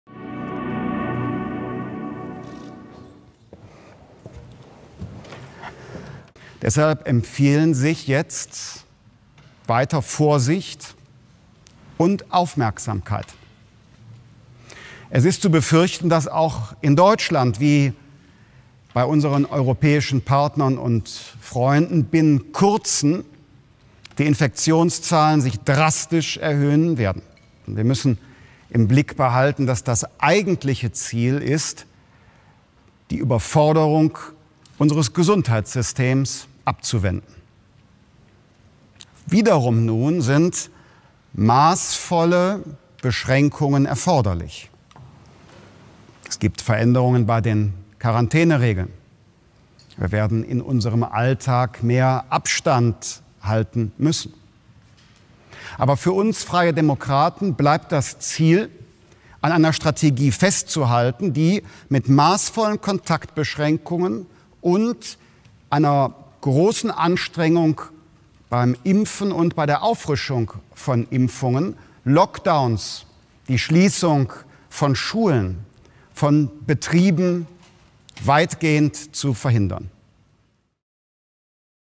Lindner bei Dreikönigstreffen: „Maßvolle Beschränkungen erforderlich“
Angesichts der befürchteten neuen Welle von Corona-Infektionen hat FDP-Chef Christian Lindner vor weitgehenden Einschränkungen gewarnt. Auf dem Dreikönigstreffen der Liberalen sagte Lindner, flächendeckende Schließungen dürfe es nicht mehr geben.